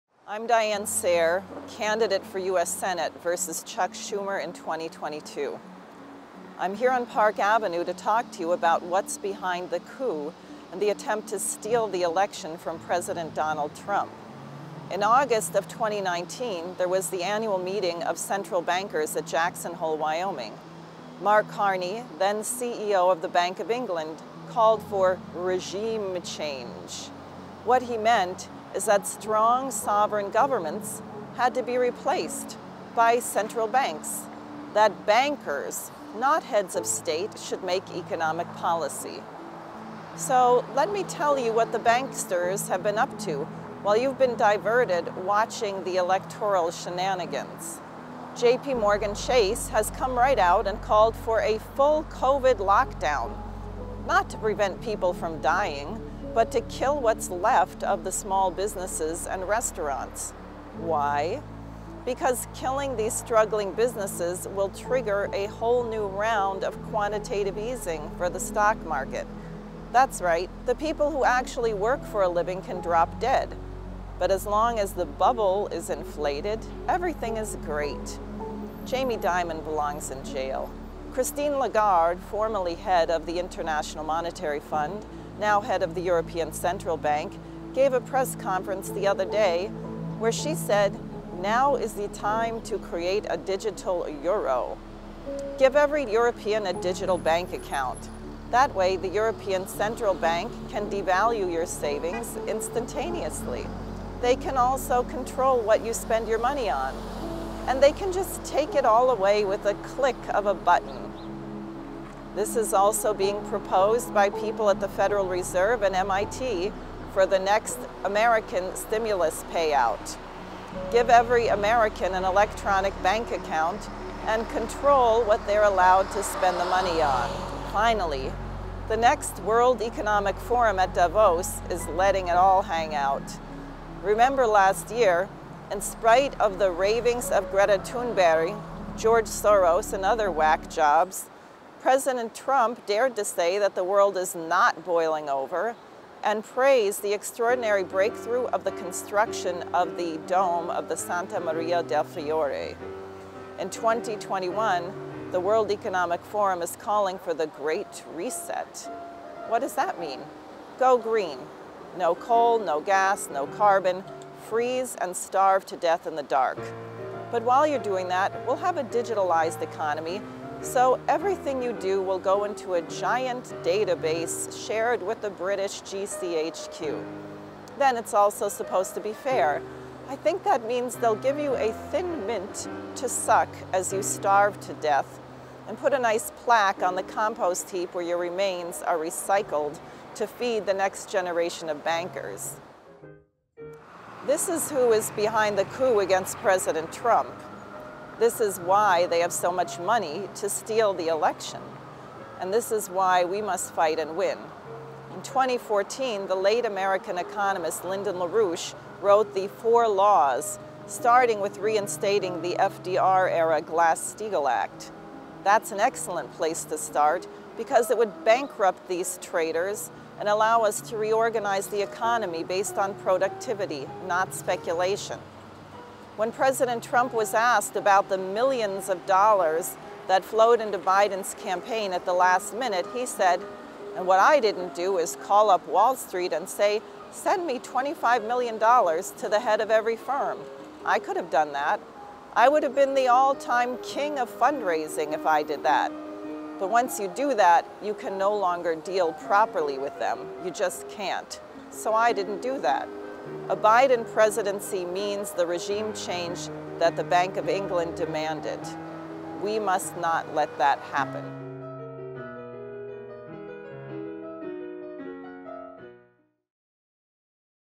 Who is STEALING the election? The BANKERS
If Biden becomes President, the USA will be under control of central bankers, not a sovereign government. There's a reason why the COVID lockdowns were forced on us and there's a reason why they want to push digital currency: to control virtually every aspect of our lives.